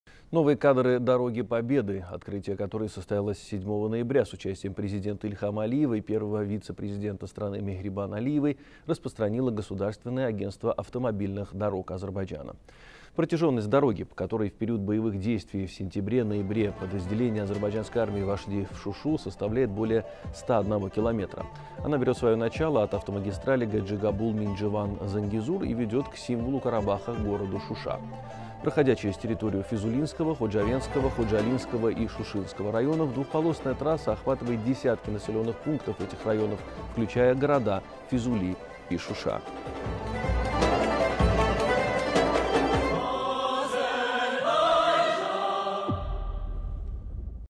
Дорога Победы к жемчужине Карабаха
Новые кадры Дороги Победы, открытие которой состоялось 7 ноября с участием Президента  Ильхама Алиева и первого вице-президента  страны Мехрибан Алиевой, распространило Государственное агентство автомобильных дорог Азербайджана.

Протяженность дороги, по которой в период боевых действий в сентябре-ноябре подразделения азербайджанской армии вошли в Шушу, составляет более 101 километра. Она берет свое начало от автомагистрали Гаджигабул-Миндживан-Зангезур и ведет к символу Карабаха – городу Шуша. Проходя через территорию Физулинского, Ходжавендского, Ходжалинского и Шушинского районов, двухполосная трасса охватывает десятки населенных пунктов этих районов, включая города Физули и Шуша.